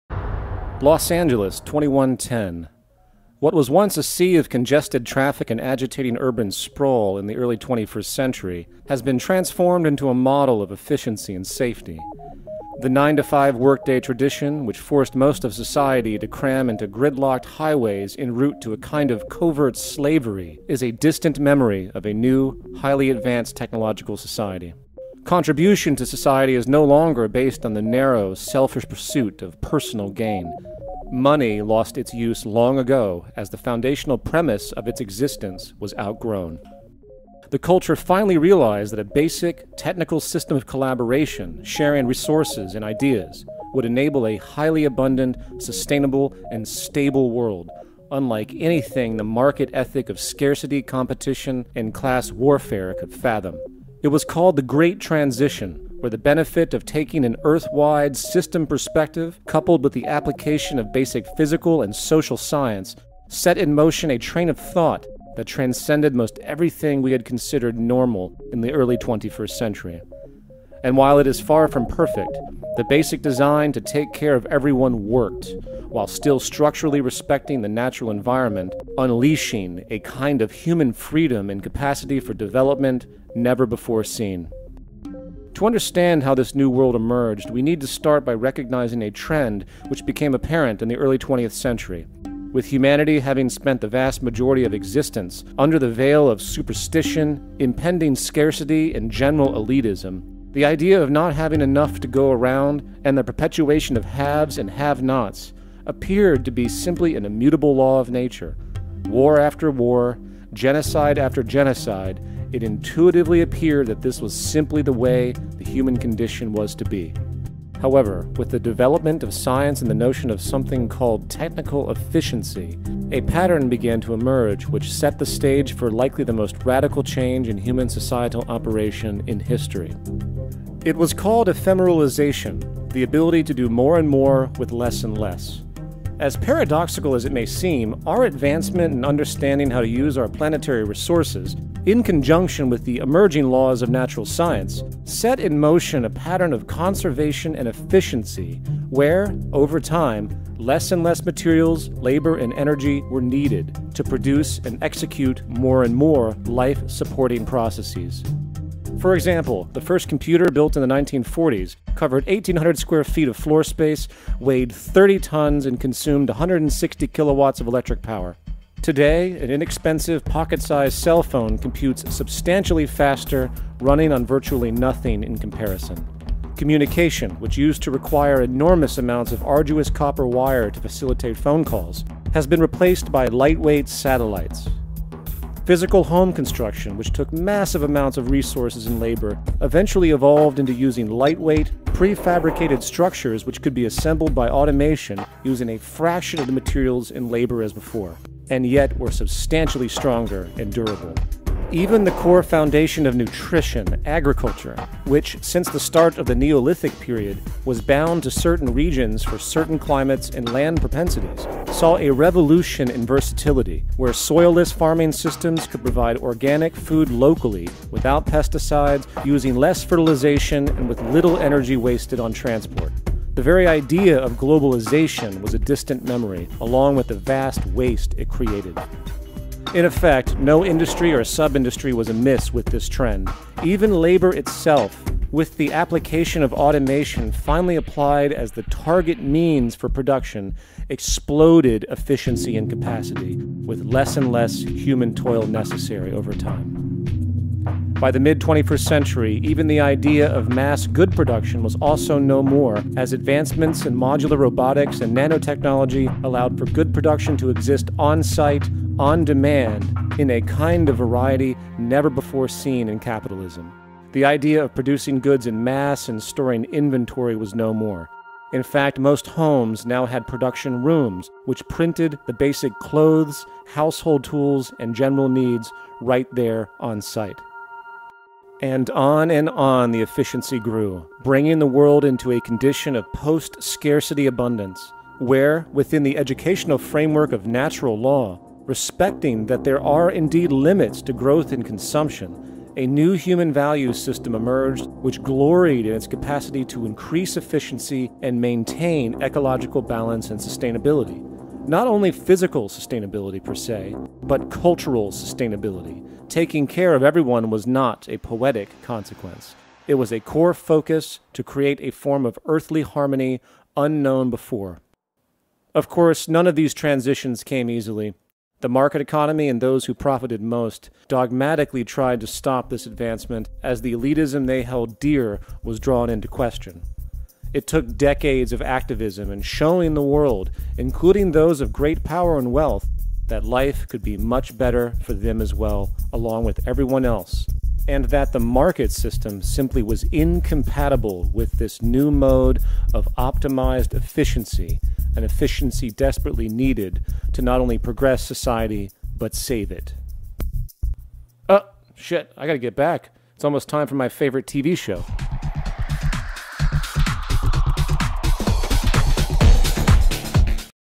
Scurta poveste a lumii viitorului: O Noua Speranta
Efemeralizarea ar fi unul din fundamentele de baza a reconstructiei societatii viitorului, avand un potential socio-economic greu de imaginat astazi. Economia bazata pe resurse ar putea fi un salt evolutiv urias catre civilizatia viitorului (vezi si Star Trek - economia viitorului)

O prezentare pozitiva a unui posibil viitor in care umanitatea ar trai intr-o lume civilizata, foarte diferita de cea actuala.